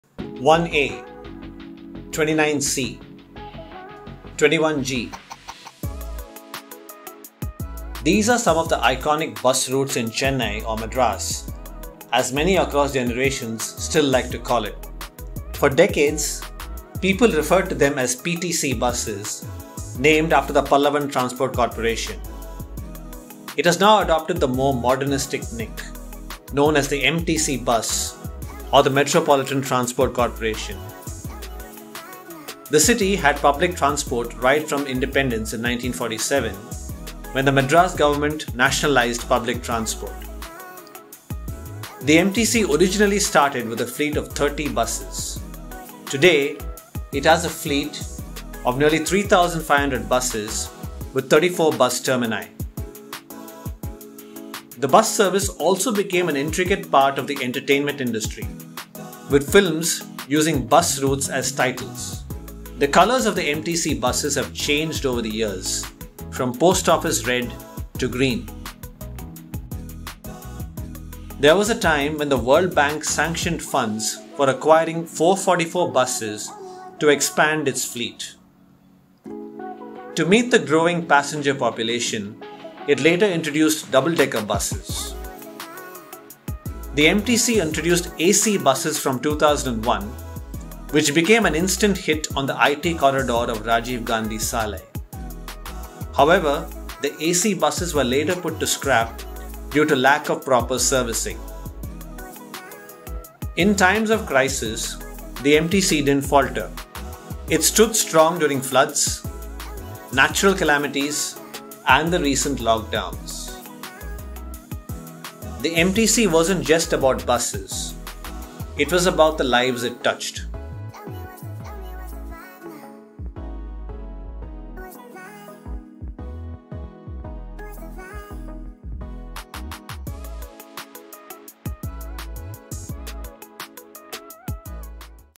Why Chennai loves its buses | Madras Day | The Hindu
1A, 29C, 21G, 12B – these are some of the iconic bus routes in Chennai or Madras.

For decades, residents referred to them as PTC buses, named after the Pallavan Transport Corporation. It has now adopted the more modernistic nick, known as the MTC bus, or Metropolitan Transport Corporation. The city had public transport right from Independence in 1947 when the Madras government nationalised public transport.

The MTC originally started with a fleet of 30 buses. Today, it has a fleet of nearly 3,500 buses with 34 bus termini.

Reporting: R. Srikanth
Video: M. Srinath
Production: Reenu Cyriac
Voiceover: Kanishkaa Balachandran